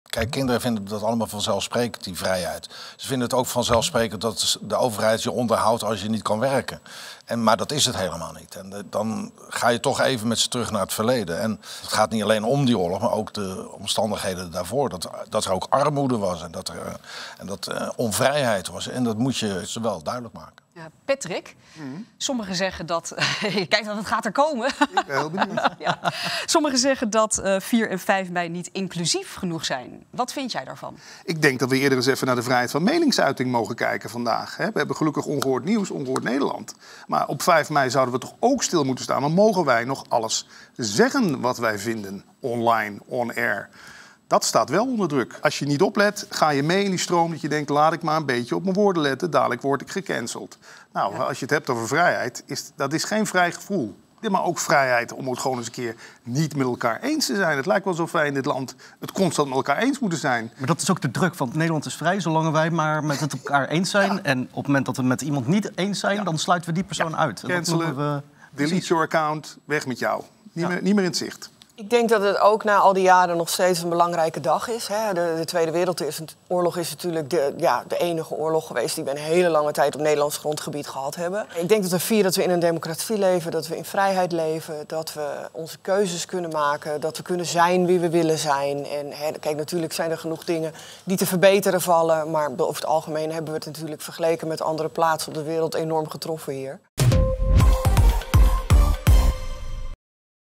'Vrijheid is niet vanzelfsprekend' - Ronald Sørensen over bevrijdingsdag
Vanaf nu zijn wij iedere dinsdag en donderdag live te zien op NPO1 met Ongehoord Nieuws.

Luister ook naar onze podcasts op Spotify:
Ongehoord Nieuwscafé: